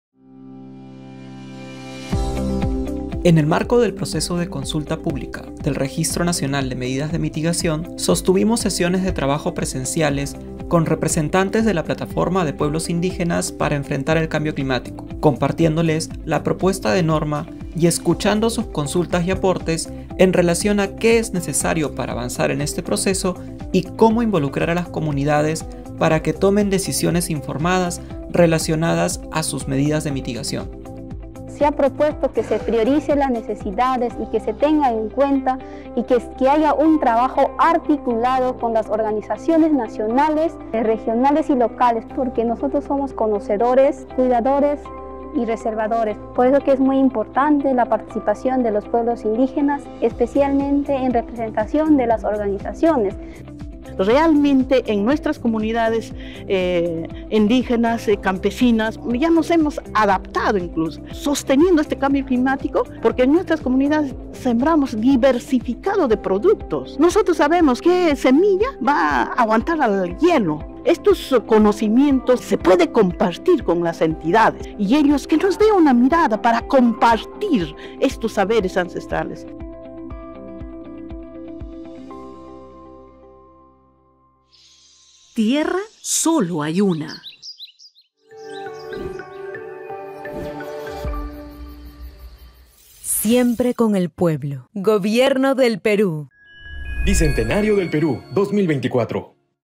Consulta pública Registro Nacional de Medidas de Mitigación (RENAMI)
📑Continua la consulta pública de la propuesta de lineamientos del RENAMI.
Realizamos sesiones informativas con diversos actores de la Plataforma de Pueblos Indígenas para enfrentar el Cambio Climático.